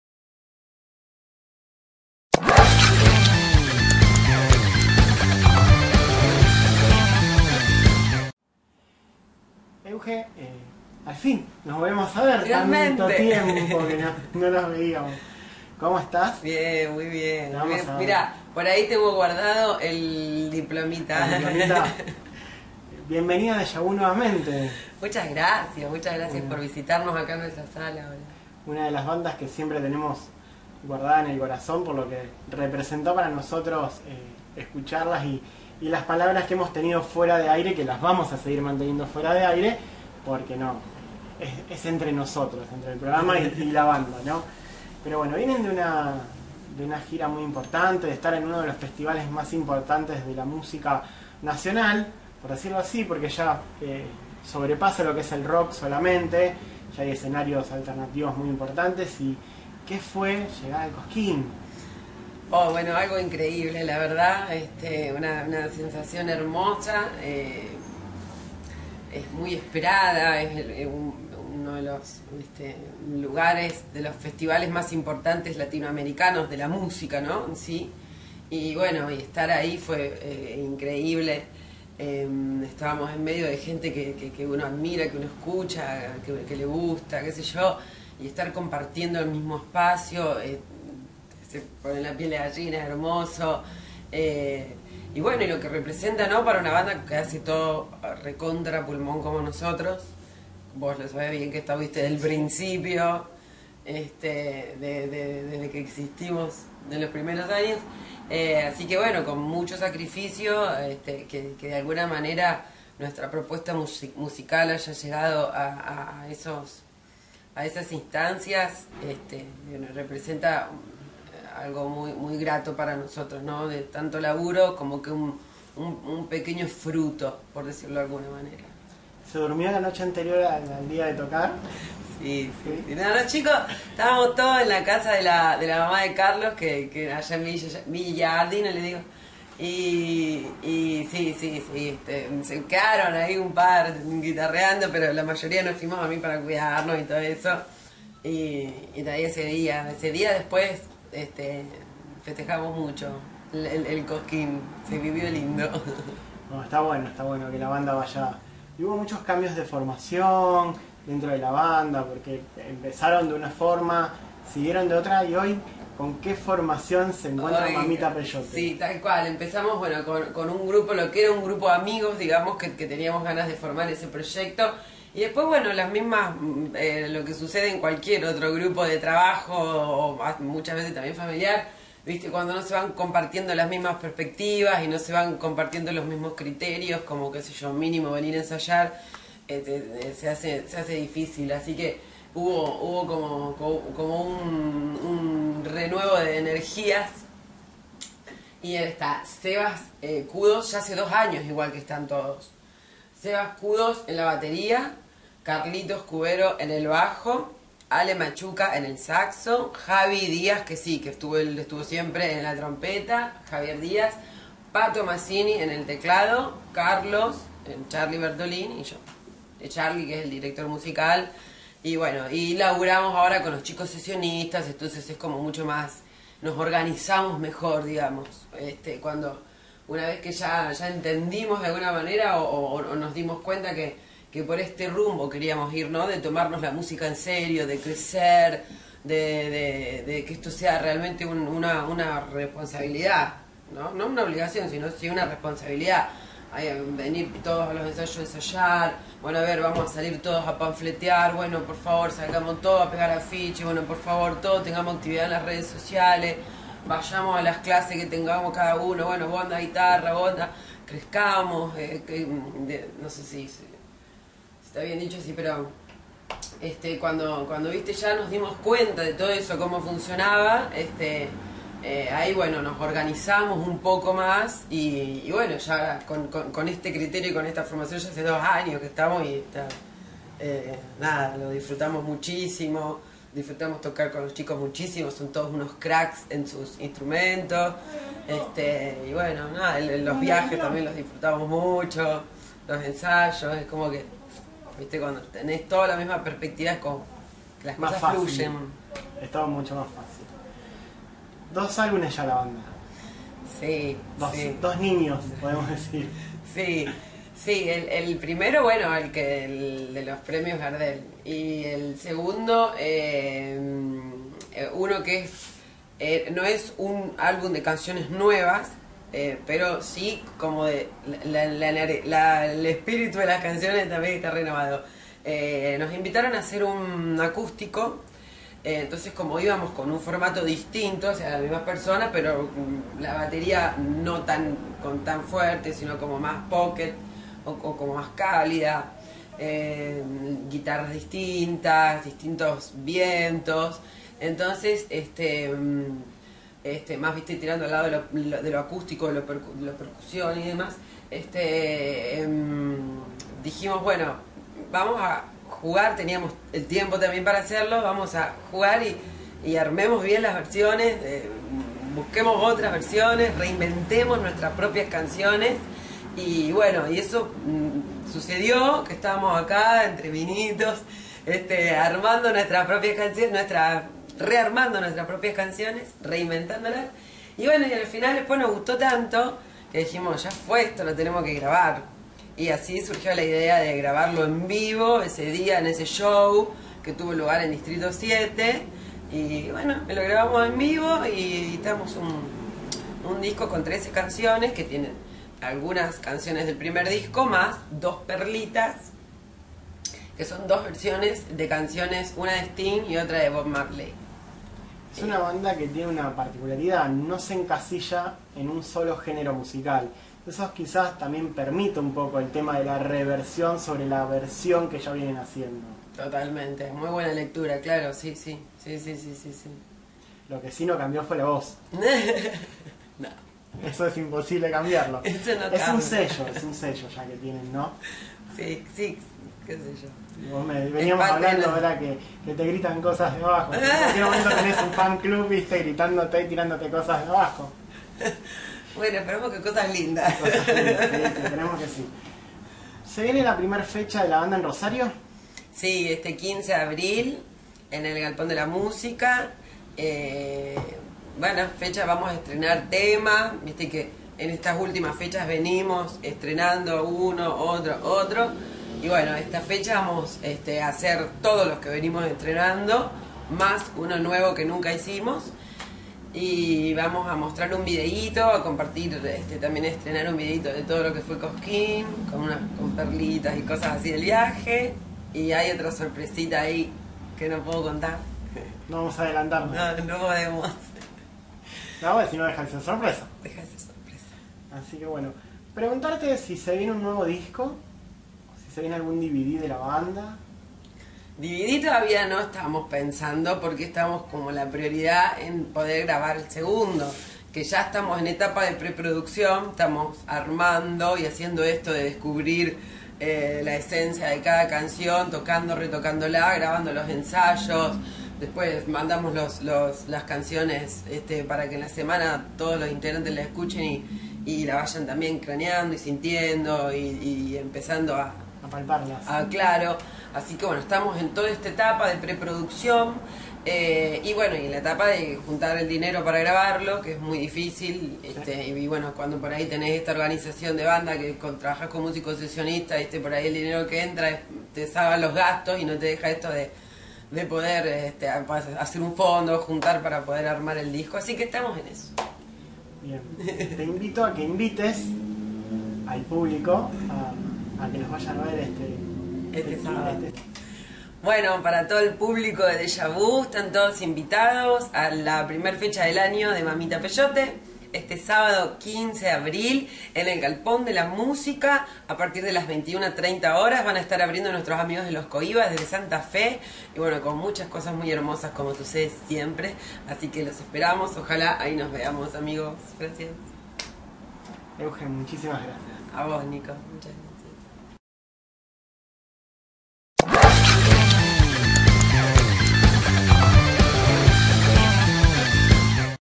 Nota a Eugenia Craviotto Carafa de Mamita Peyote
Eugenia Craviotto Carafa vocalista de Mamita Peyote nos adelanto el show de la banda este 15 de abril en el Galpón de la Música